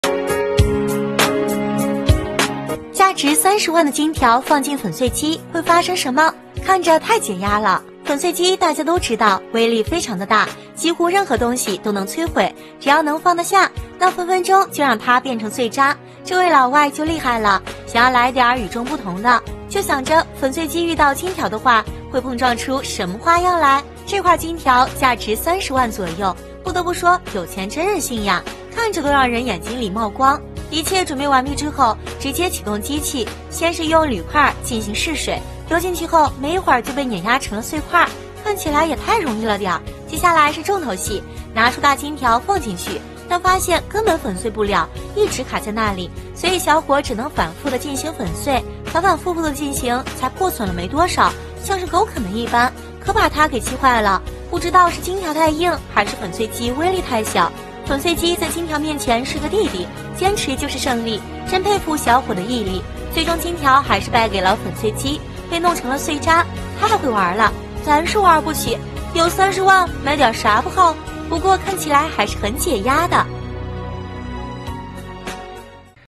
价值30万的金条放进粉碎机，会发生什么？看着太解压了！
价值30万的金条放进粉碎机，会发生什么？看着太解压了！,本视频由科技道道客原创提供,154次播放,好看视频是由百度团队打造的集内涵和颜值于一身的专业短视频聚合平台